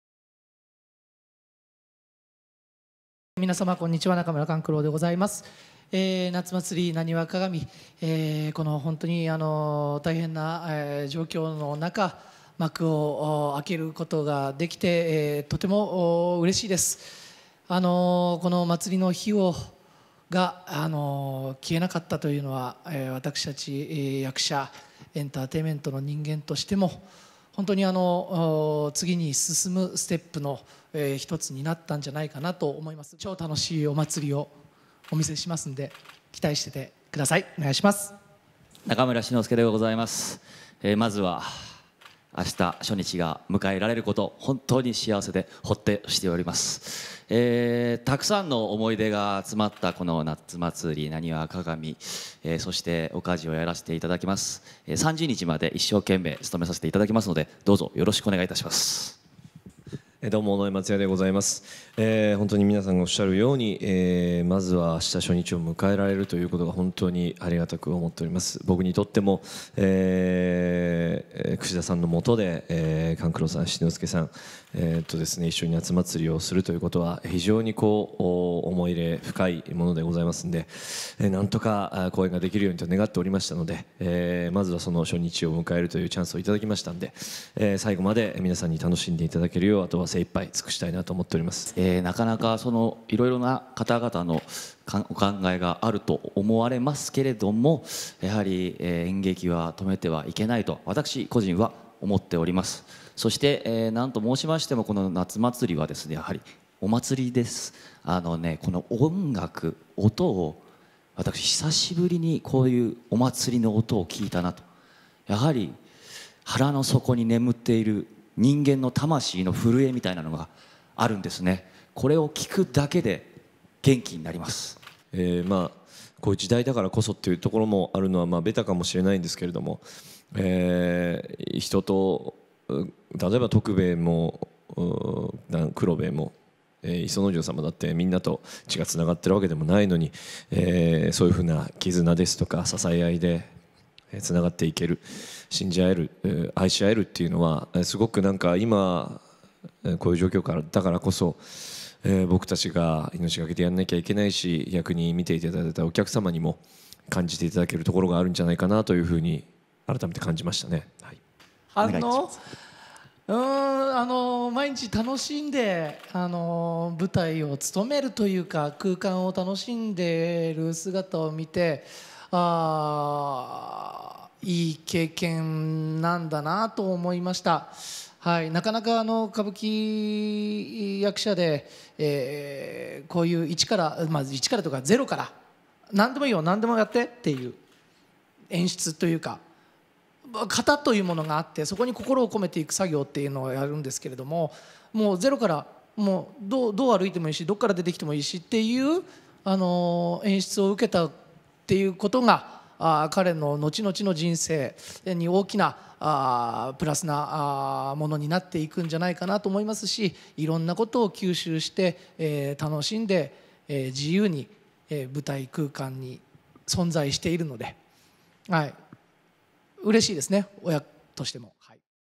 中村勘九郎、七之助、尾上松也が意欲を語る コクーン歌舞伎 第十七弾『夏祭浪花鑑』取材会
歌舞伎俳優の中村勘九郎、中村七之助、尾上松也が11日、オンラインで行われた渋谷・コクーン歌舞伎 第十七弾『夏祭浪花鑑』の取材会に出席した。

演出家の串田和美氏と18代目・中村勘三郎がタッグを組んで1994年に誕生したコクーン歌舞伎。浪花の市井の人々を描いた『夏祭浪花鑑』は、96年、2003年、08年に上演されている人気演目で、アメリカやヨーロッパでの公演も成功を収めている。

コロナ禍でありながら、13年の時を経て公演することに勘九郎は「休業要請でお店が開けられない方がいらっしゃる中、エンターテイメントが幕を開けられるのは幸せなことですが、お客さまの安全が第一です。安全安心に見られて、苦しい日常を少しでも忘れていただける舞台空間をお届けしたい。生きていることを届けるのが使命で公演の意義だと思っています。生きることを味わっていただけたら」と言葉を紡いだ。

七之助は「いろいろなお考えがあると思いますが、演劇は止めてはいけないものと思っております。『夏祭』はお祭りです。久しぶりにお祭りの音を聞きましたし、腹の底に眠っている人間の魂の震えみたいなものがある。これを聞くだけで元気になります。安心安全に公演は行われておりますので、ぜひお足をお運びください」とファンにメッセージを送った。

松也は「自分も自粛しているときに、エンターテイメントに演劇に救われたことがございました。チャンスをいただけたなら、お客さまにとって希望になるようなお芝居ができたら」と話した。

公演は、東京・Bunkamuraシアターコクーンにて、あす12日から30日まで行われる。